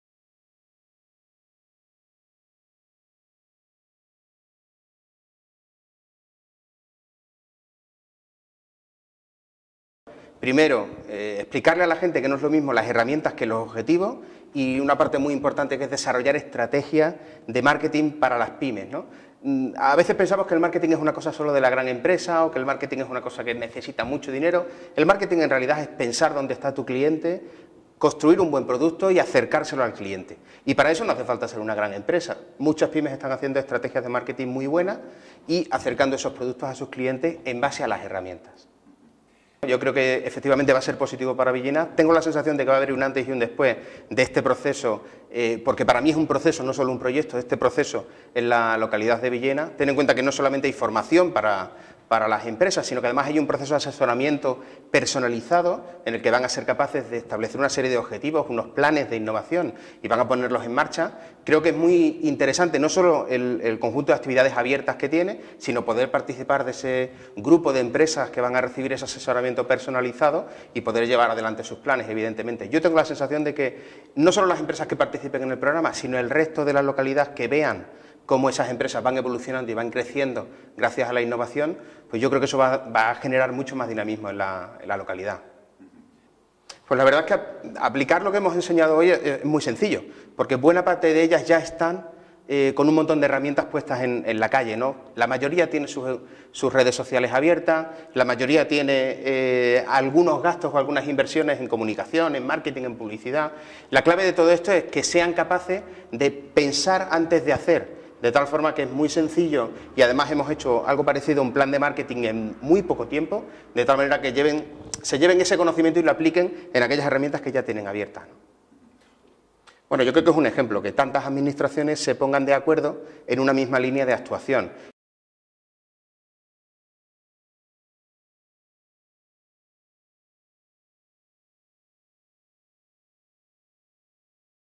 Entrevistamos a David Sánchez en el CdE Villena
David Sánchez, psicólogo, coach empresarial y ejecutivo, experto en estrategias de marketing y comunicación, y director de desarrollo corporativo de la empresa PayPerThink. David Sánchez lleva más de 15 años ayudando a los empresarios a crear sus negocios y darles mayor visibilidad. Dirigió durante 5 años la comunicación y relaciones institucionales de CENATIC, Centro Nacional de Referencia del Software Libre del Ministerio de Industria, Energía y Turismo. Experto nacional de la Fundación Banesto para el Emprendimiento, y de EULEN en materia de proyectos turísticos, David Sánchez es en la actualidad presidente de AEXCO, la Asociación Extremeña de Coaching.